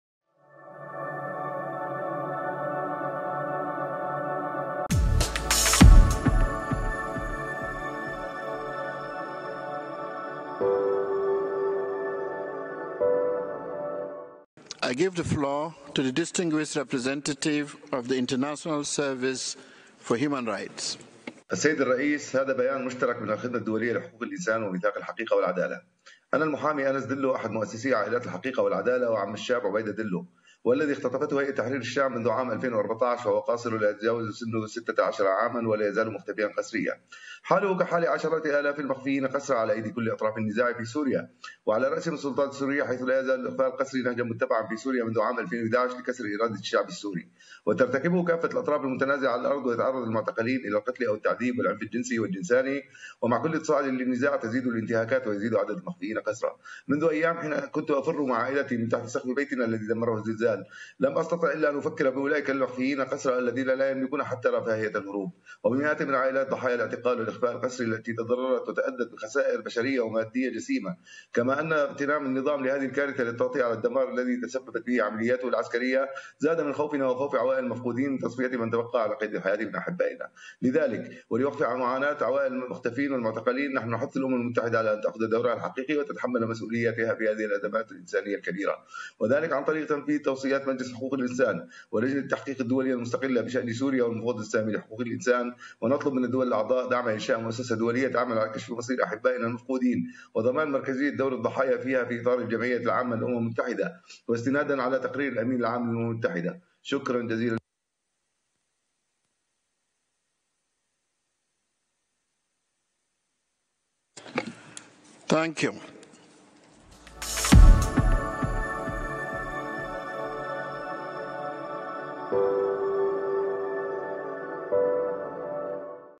HRC52: families of forcibly disappeared Syrians call for mechanism on the fate of the missing
During the Interactive Dialogue with the Commission of Inquiry on Syria under agenda item 4, ISHR and the Truth and Justice Charter Group delivered a joint statement addressing the continued suffering of families of forcibly disappeared Syrians at the hand of all parties to the conflict in Syria. Speaking on behalf of a coalition of 10 associations of survivors and families of individuals disappeared by all parties to the conflict, Anas Tello addressed the case of his nephew, Obaida Dello, who remains forcibly disappeared since 2014 following his kidnapping by the Hay'at Tahrir al-Sham.